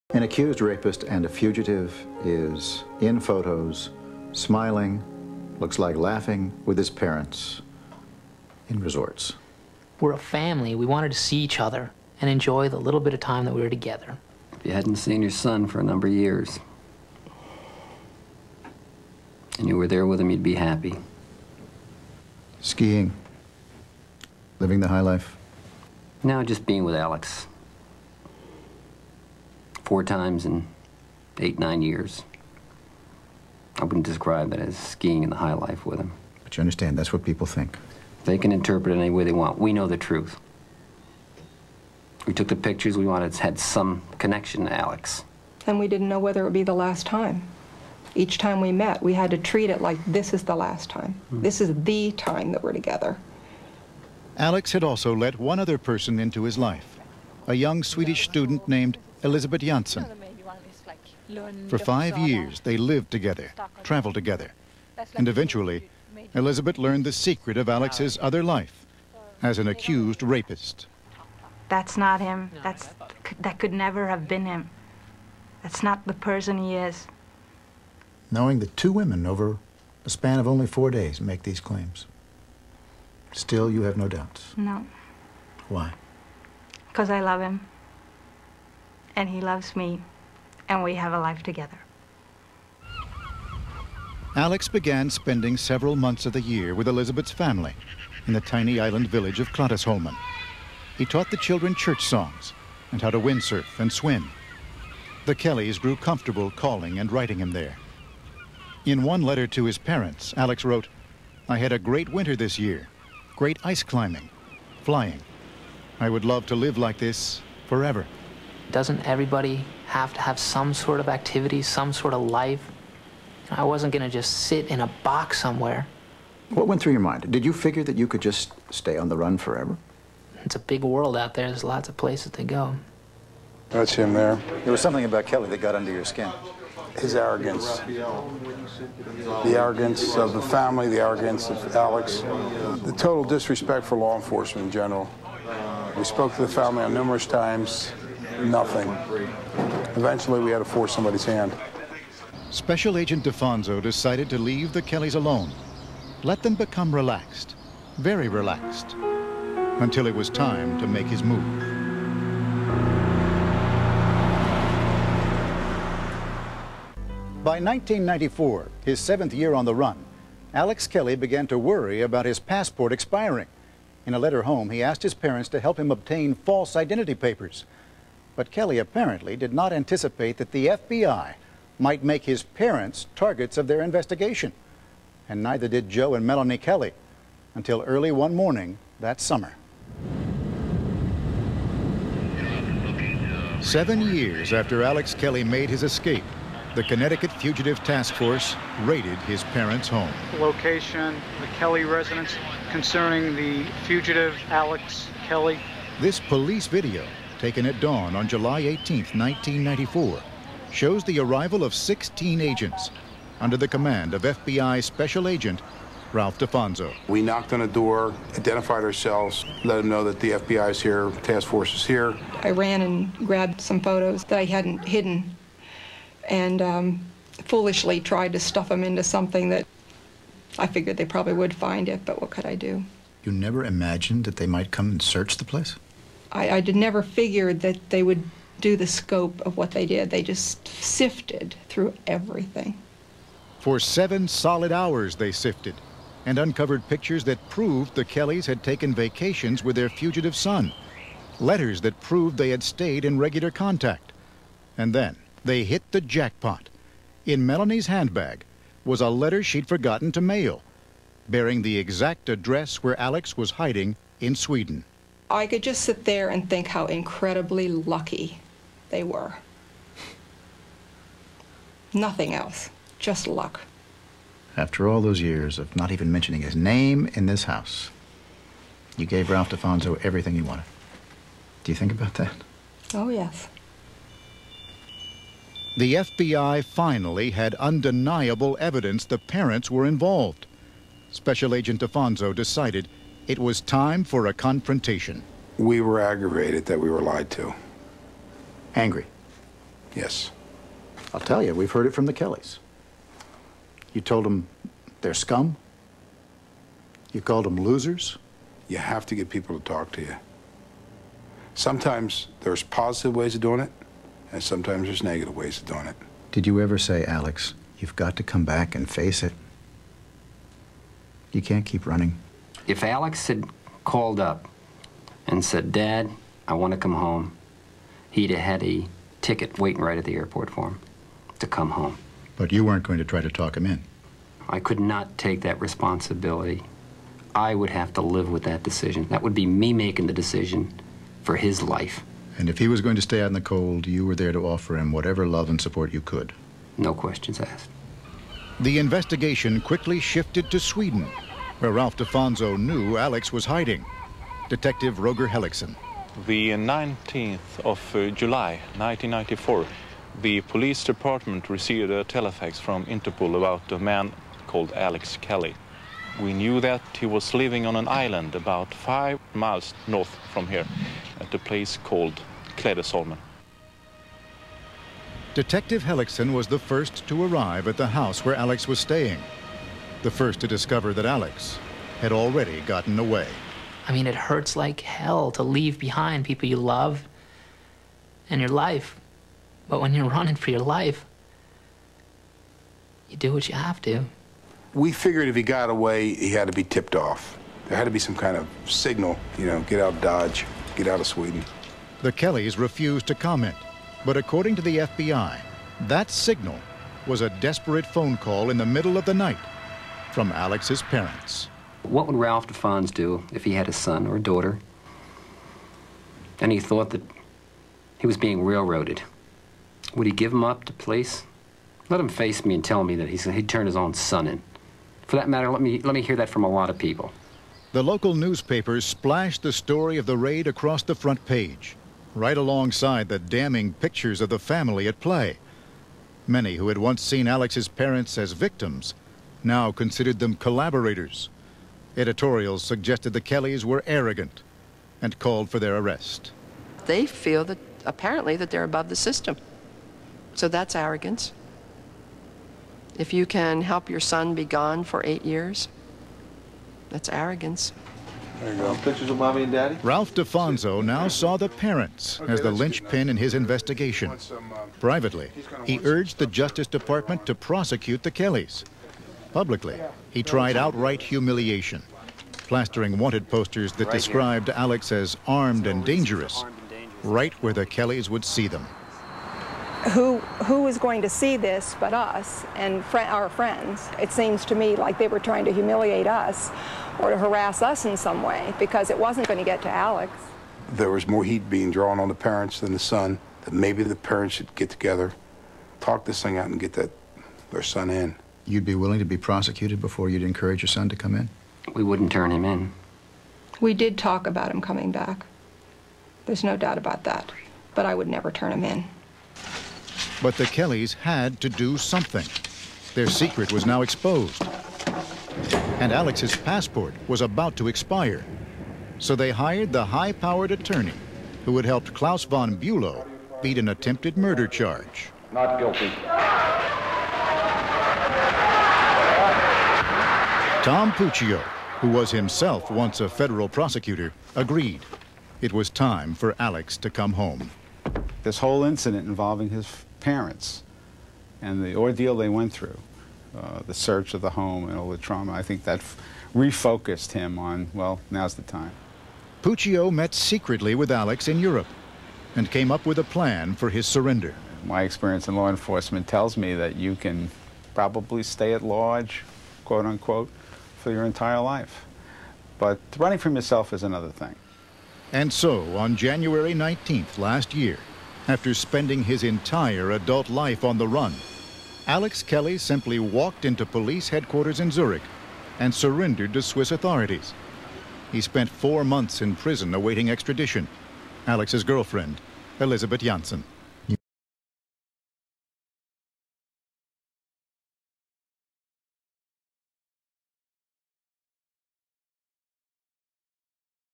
Accused Preppy Rapist Alex Kelly Interviewed on ABC - 4
Shortly before his long overdue 1997 trial for rape, Alex Kelly and his parents were interviewed by ABC's Forrest Sawyer. The talk concentrates on the Kellys' decision to support their son while he was a fugitive from justice hiding in Europe for several years. Additional information can readily be found on the web, starting with the Wikipedia entry